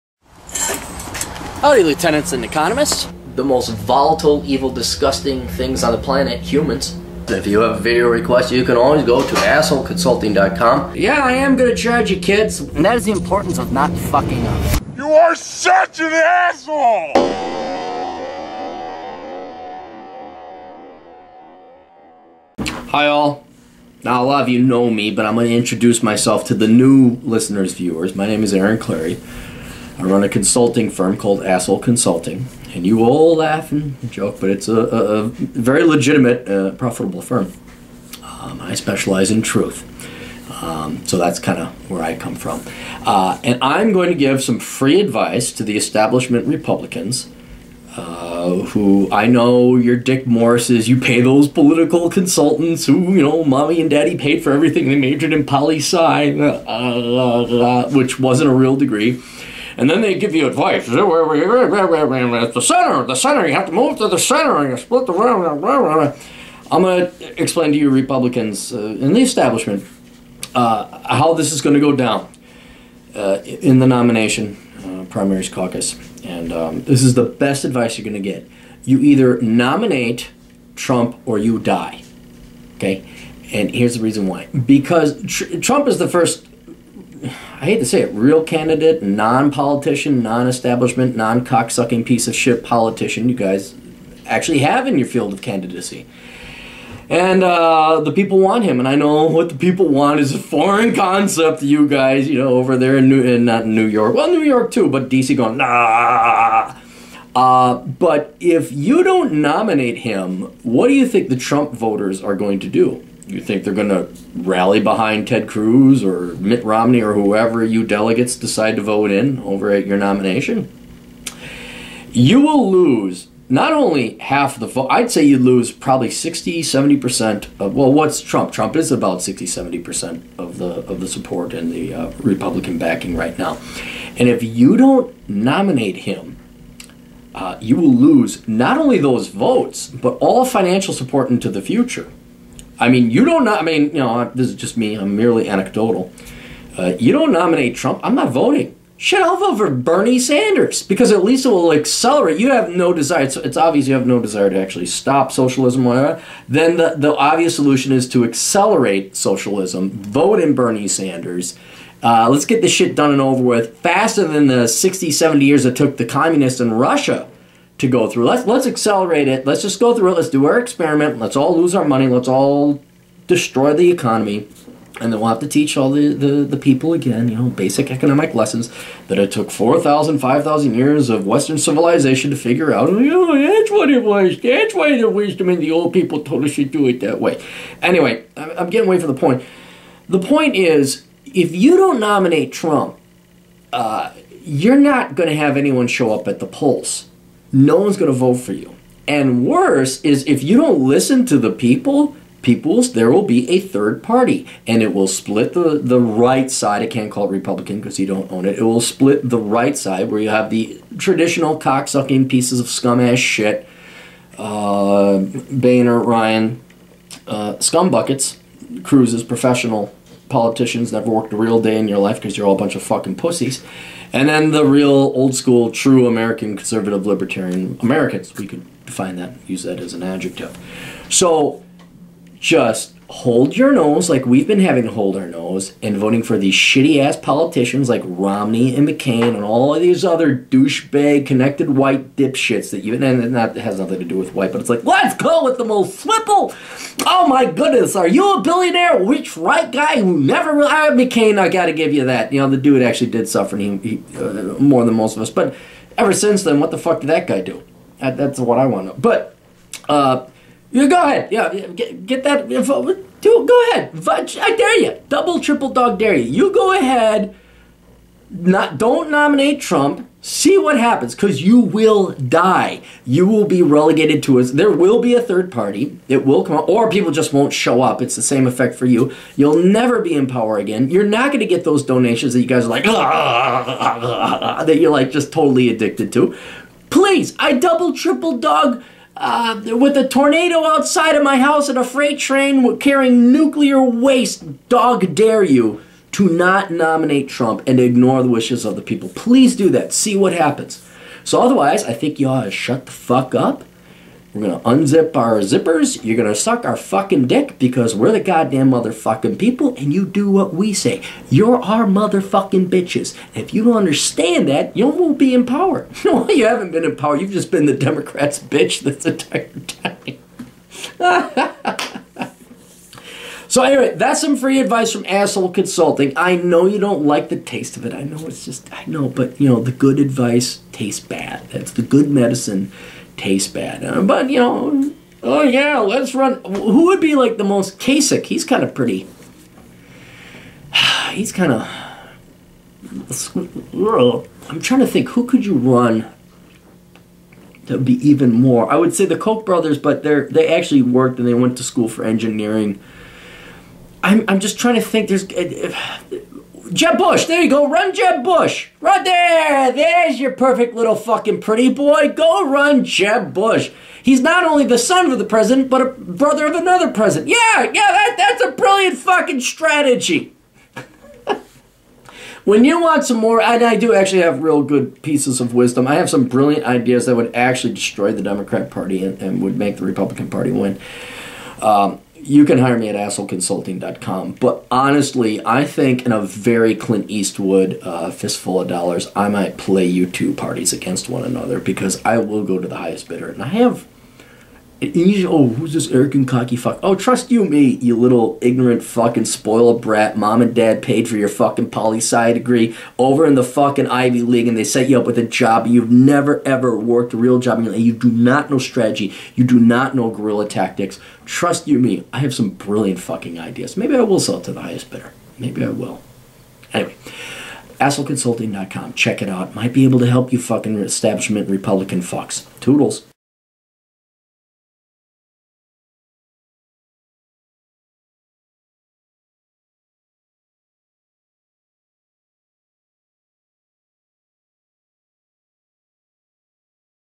Vote Trump or Die
It's very simple.  If Trump doesn't get the nomination, nobody will show up to vote and the democrats will win.  So vote Trump or die.